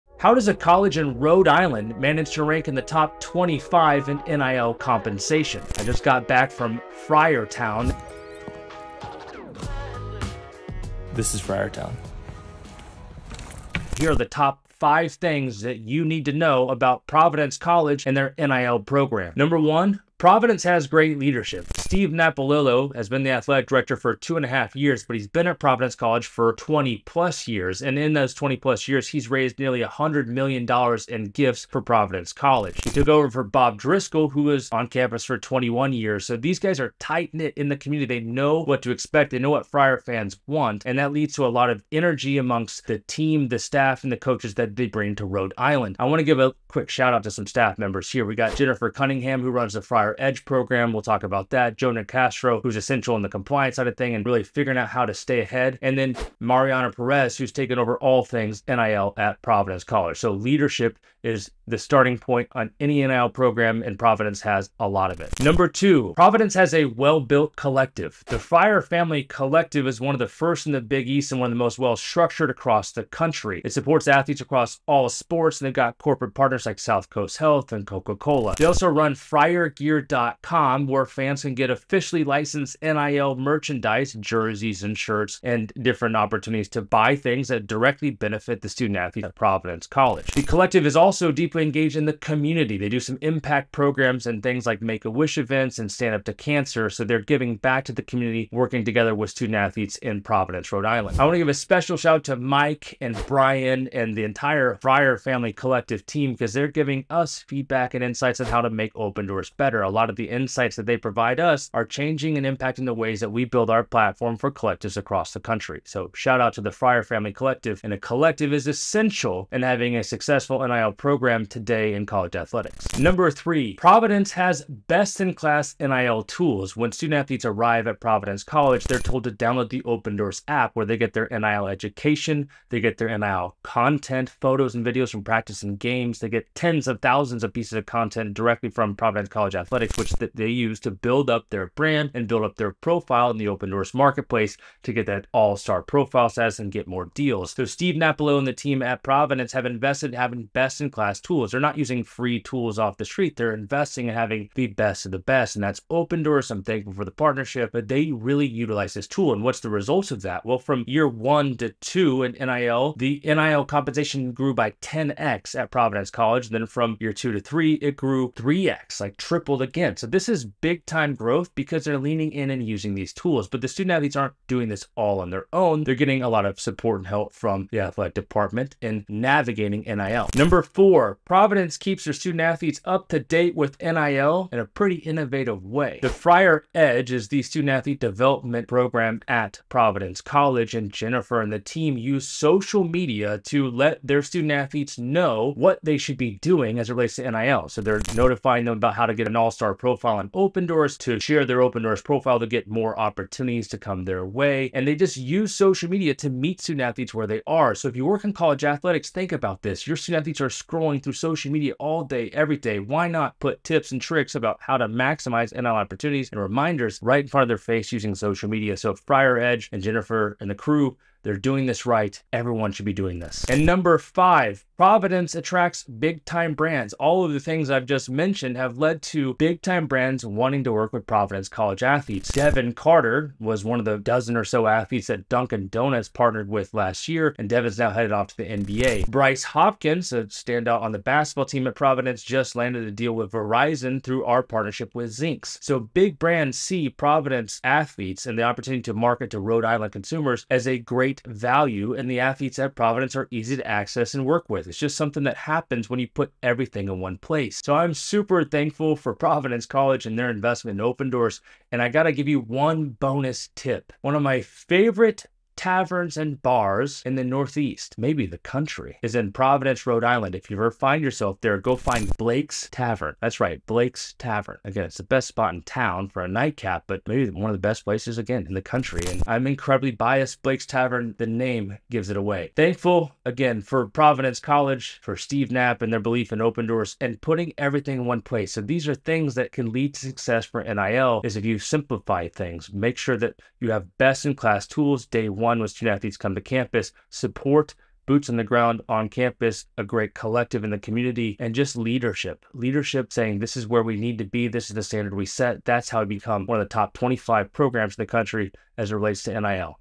How Providence College became a Top 25 NIL program
How does a college in Rhode Island manage to rank in the top 25 for NIL compensation? I just got back from FRIARTOWN and here are the top 5 things you need to know about Providence College and their NIL success.

1. Providence Has Great Leadership:
+ Athletic Director Steve Napolillo has secured nearly $100 million in gifts and has a dedicated team including Jennifer Cunningham, Joe Nicastro, and Mariana Perez.

2. Providence Has a Well-Built Collective:
+ The Friar Family Collective supports athletes with partners like South Coast Health, Amica, and Coca-Cola. They also run FriarGear.com for NIL products.

3. Providence Has Best-in-Class NIL Tools:
+ Athletes get access to Opendorse for NIL education and deals. Their NIL compensation has grown exponentially, ranking them in the top 25 among NCAA Division I schools.

4. Providence Keeps Athletes Up-to-Date on NIL:
+ The Friar Edge program uses social media to inform athletes about NIL opportunities, helping them maximize their potential.

5. Providence Attracts Big-Time Brands:
+ Athletes like Devin Carter and Bryce Hopkins have secured deals with Dunkin Donuts and Verizon.

Bonus Tip:
+ Visit Blake’s Tavern in Providence, my favorite pub in the Northeast.

Providence College is reshaping the collegiate athletic experience with innovative programs, strong community support, and cutting-edge technology. Their NIL program is truly impressive.